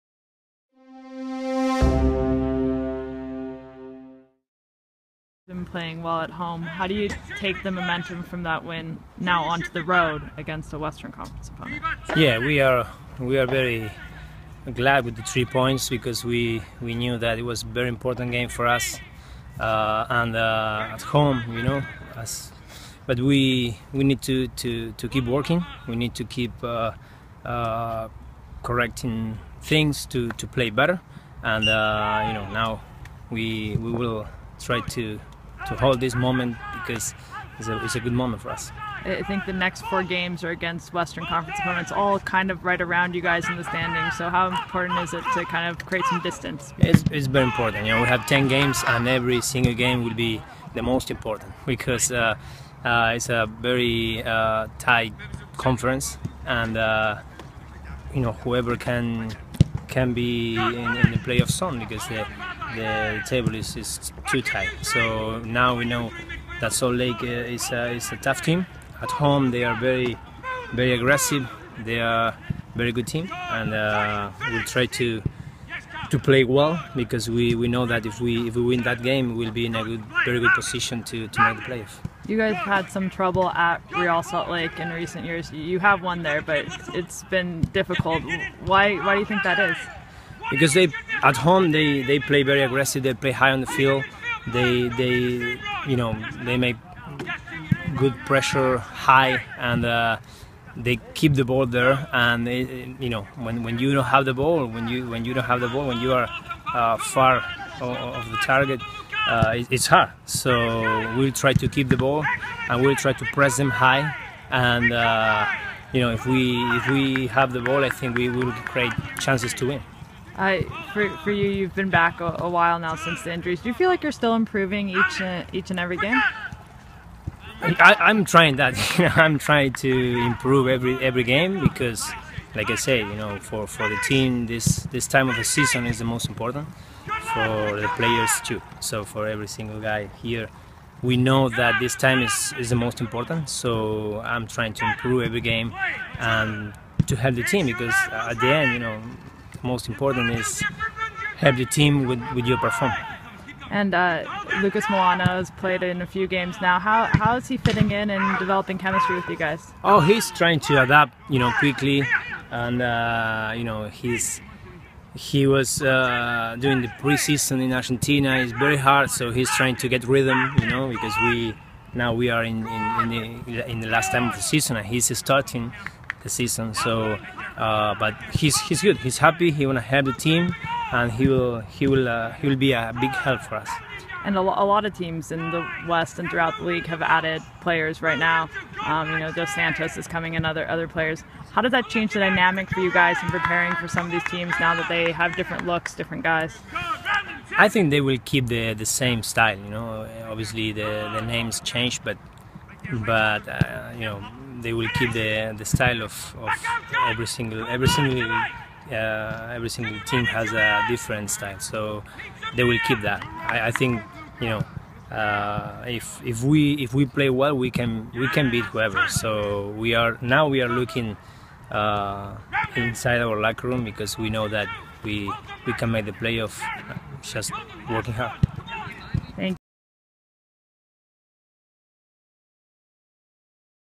Portland Timbers midfielder Diego Valeri on Saturday's game against Salt Lake: Video
Portland Timbers midfielder Diego Valeri talks about Saturday's game against Real Salt Lake.